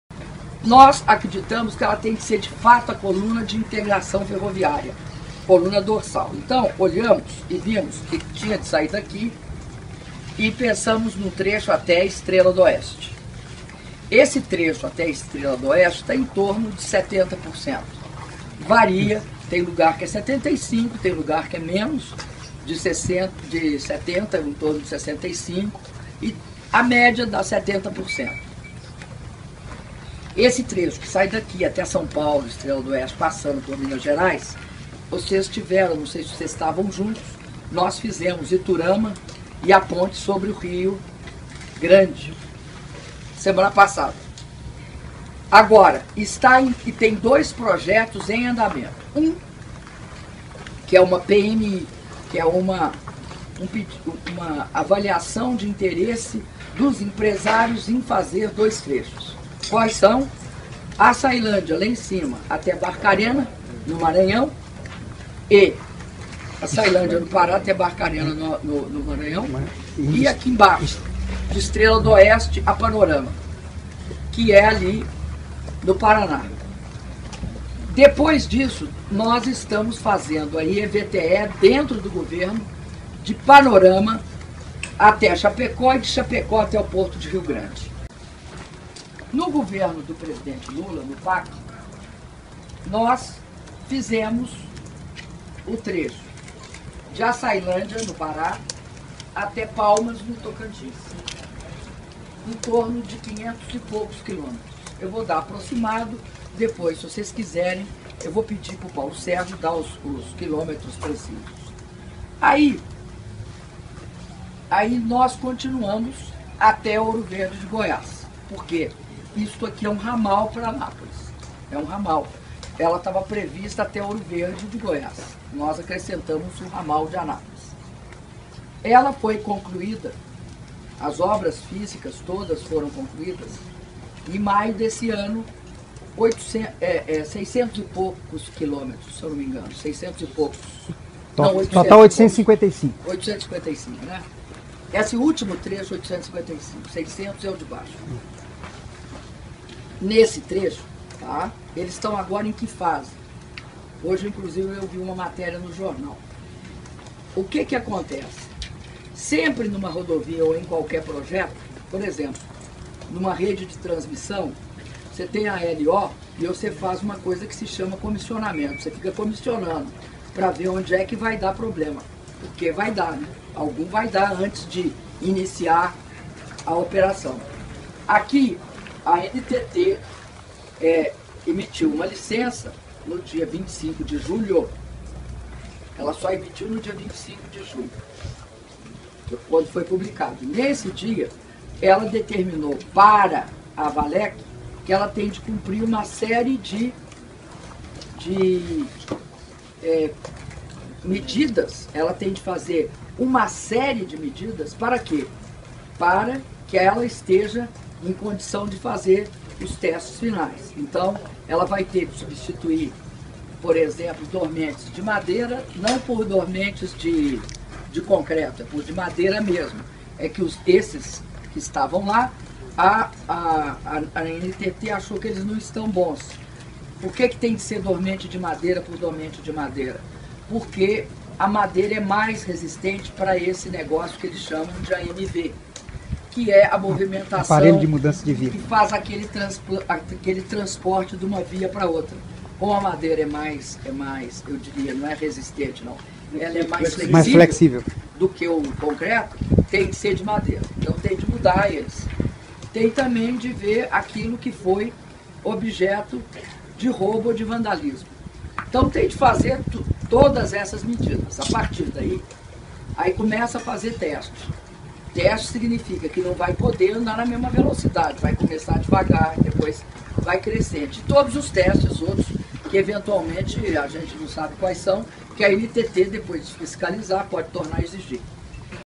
Dilma em Anápolis: coletiva de imprensa 12.08
Em visita ao trecho da Ferrovia Norte-Sul em Anápolis (GO), Dilma fala sobre a importância da ferrovia que será a "espinha dorsal" da produtividade brasileira.